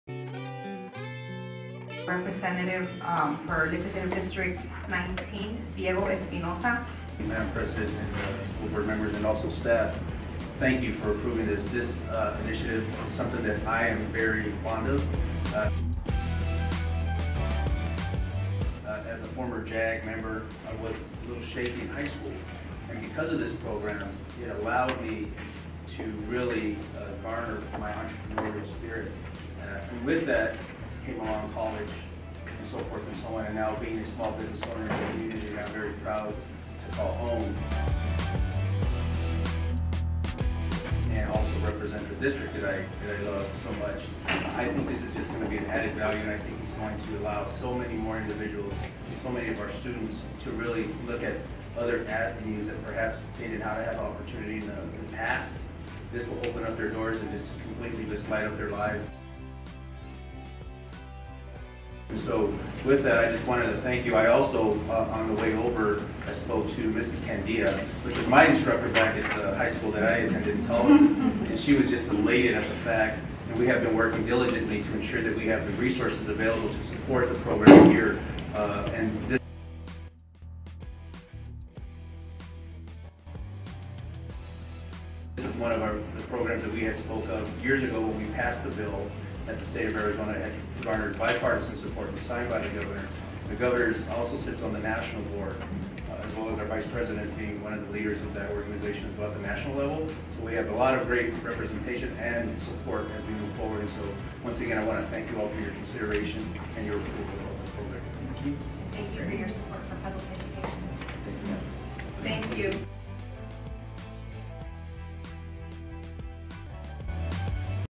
Representative Diego Espinoza shares JAG
The Agua Fria Union High School Governing Board passed
the initiative to implement the JAG program at
Agua Fria High School. The program will launch the
Fall of 2020! Check out this video clip as Rep. Espinoza shares about JAG at that meeting.

Thank you, Rep. Espinoza for supporting JAG and the students we serve!